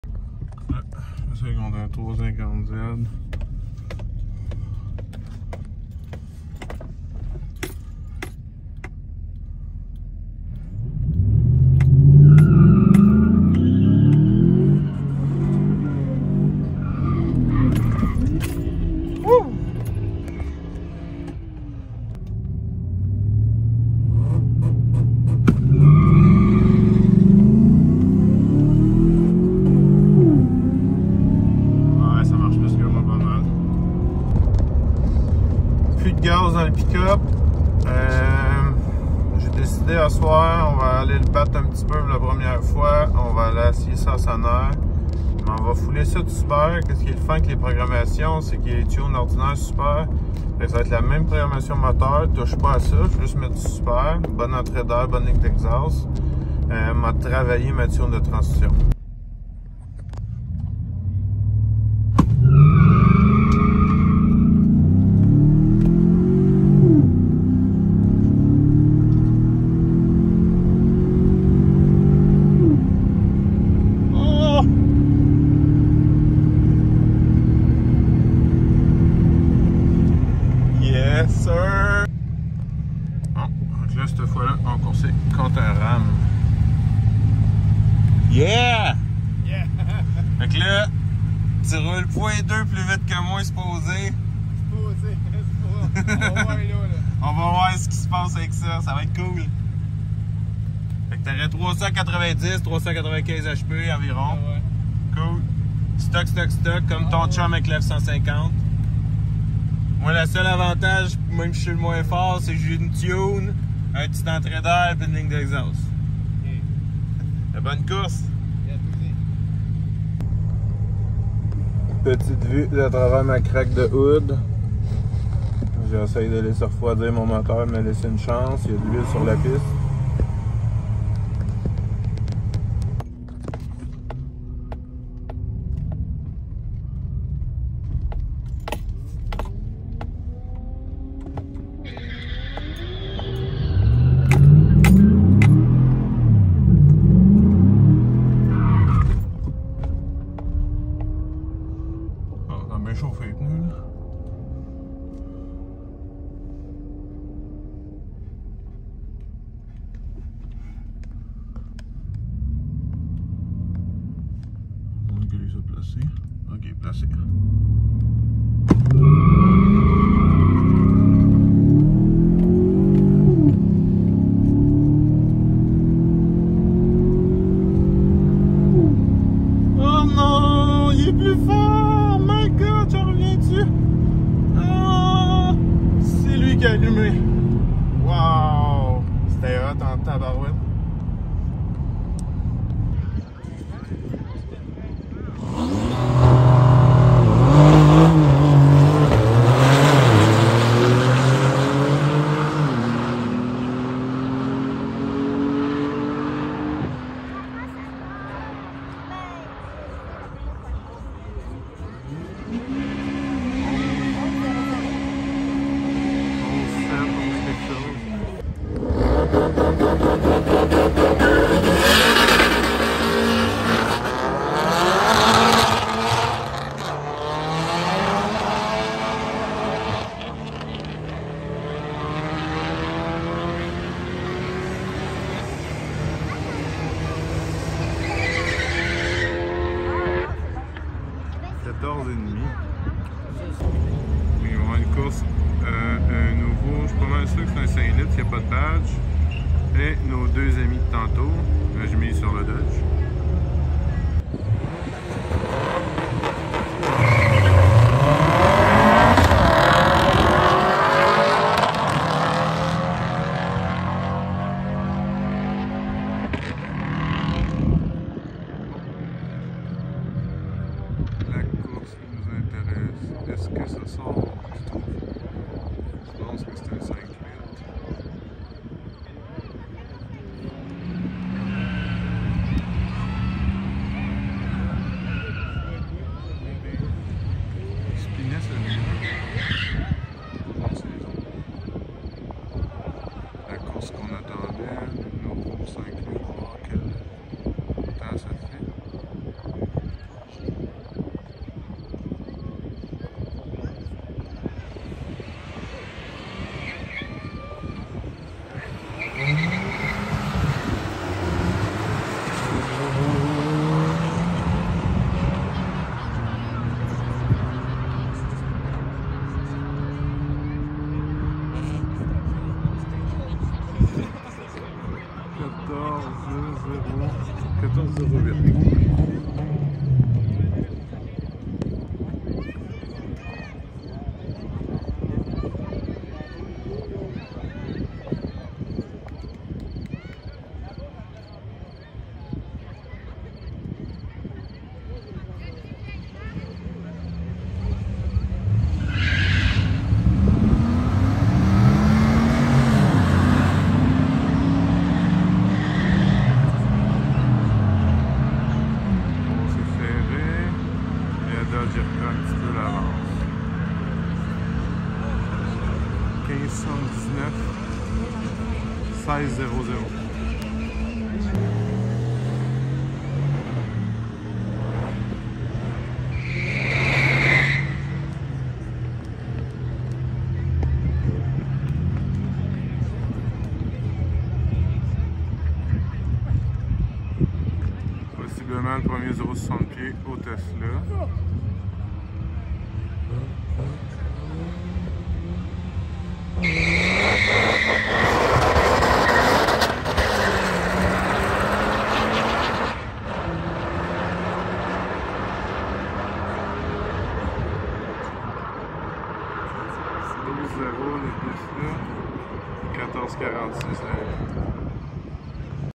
Je test mon Sierra a Sanair
Sierra 5.3 6 vitesses 2018, Tuning Hp Tuners mobile , cold air afe momentum gt , ligne exhaust magnaflow street serie